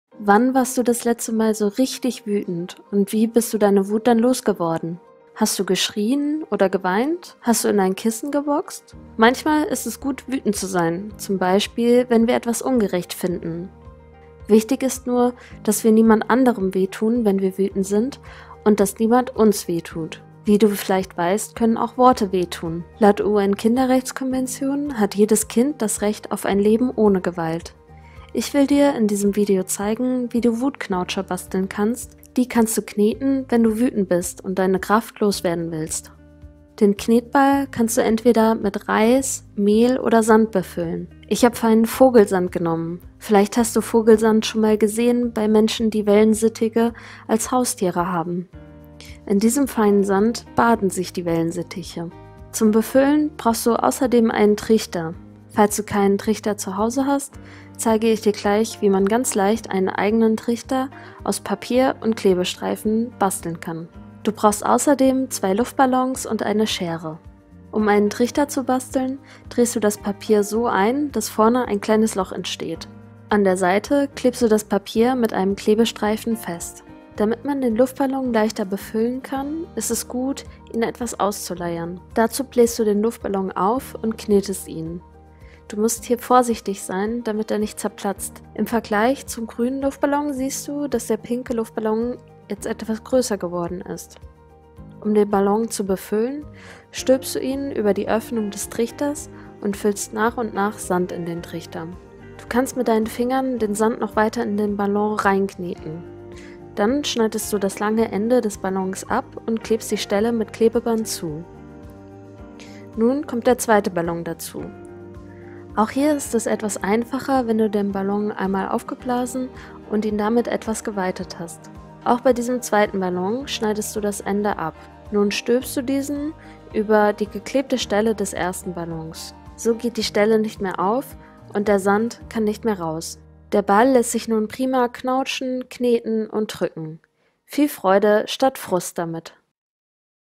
Wutknautscher - MACHmit! Werkstatt für Zuhause
Wann warst du das letzte Mal so richtig wütend? Und wie bist du deine Wut dann los geworden? Wichtig ist, dass wir niemandem weh tun und uns niemand weh tut. - Jedes Kind hat das Recht auf ein gewaltfreies Leben (Artikel 19 der UN-Kinderrechtskonvention). Ein Wutknautscher kann dir helfen, wenn du das nächste Mal wütend bist und du deine Kraft los werden willst.